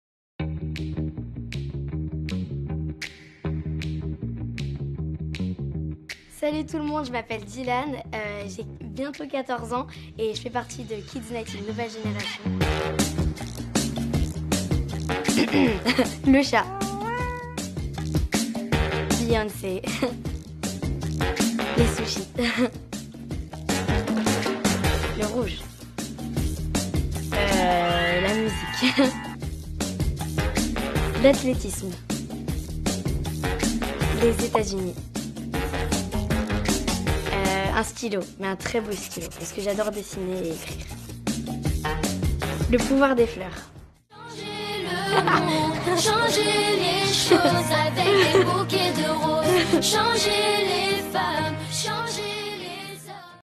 Et si tu étais ... ? - Dylan réponds à toutes les questions !!!!!
*Correction* : Dylan est un GARCON pour ceux qui demande !

Désoler a tout ceux que j'ai dit l'inverse mais franchement je vous jure je vient de l'apprendre !! J'ai toujours cru que c'était une fille , Meme sur le site des kids united y'a un moment y'a écrit "Elle" en parlant de Dylan et en plus il ressemble beaucoup à une fille :-)